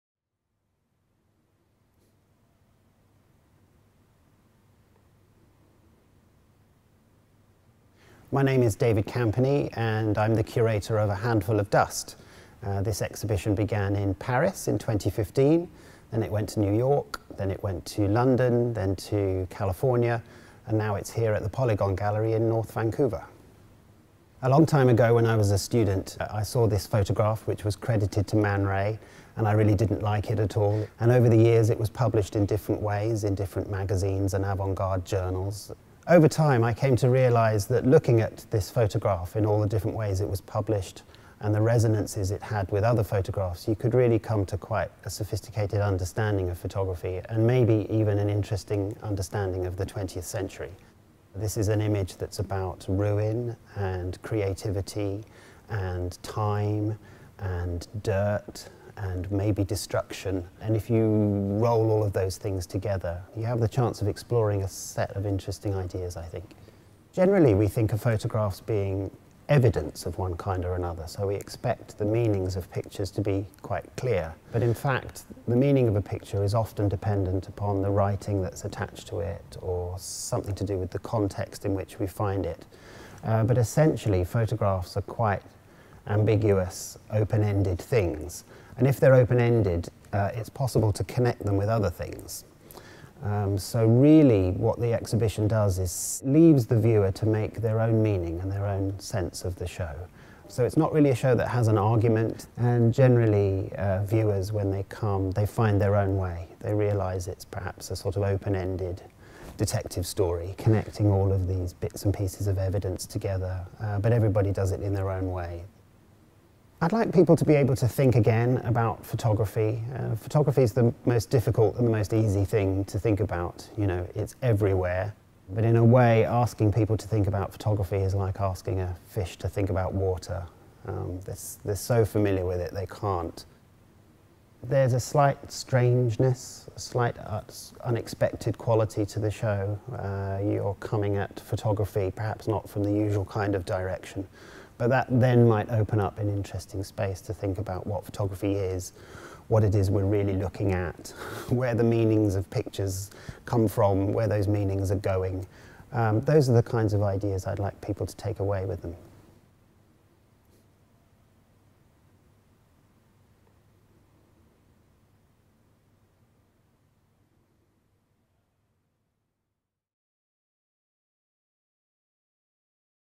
a Handful of Dust: from the Cosmic to the Domestic
'a Handful of Dust' offers a rare opportunity to view a remarkable diversity of photographs from the last 100 years, focusing on the theme of dust and debris as an indicator of time, action, and consequence.

From aerial reconnaissance and forensics, to conceptual art and abstraction, more than 30 artists and photographers are featured, some of which include Marcel Duchamp, Man Ray, Louise Albin-Guillot, Walker Evans, Brassai, Scott McFarland, Gerhard Richter, Mona Kuhn, Sophie Ristelhueber, Eva Stenram, Jeff Wall, and Nick Waplington. Their works are shown alongside anonymous press photos, postcards, magazine spreads and movies.

Documentation of programming is made possible through the generous support of Yosef Wosk, OBC.

a Handful of Dust
February 8, 2019 — April 28, 2019